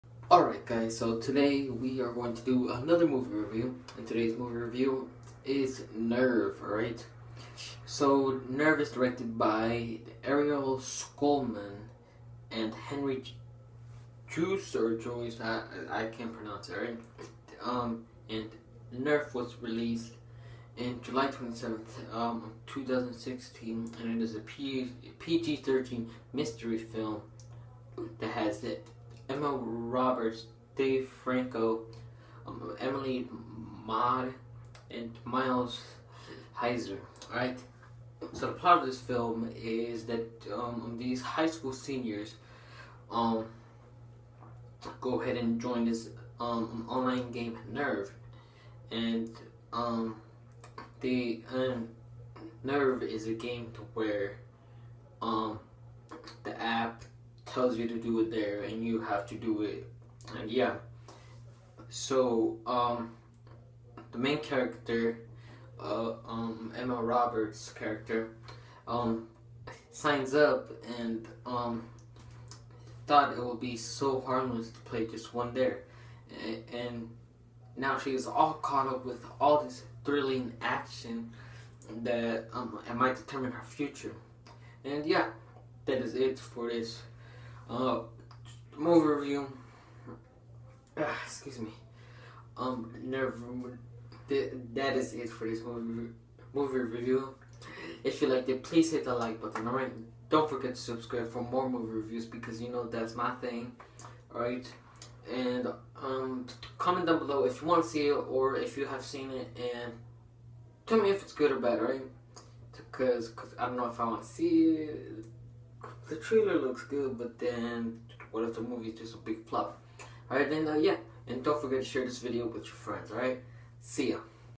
Movie review: nerve
Nerve is a film about an app and dares it stars Emma Roberts and Dave Franco go see it in theaters today

Wow guys!!! You have finished. Awesome!!! Make to like the video. Comment down What You Thought of it.

Musical.ly:jinx_hybrid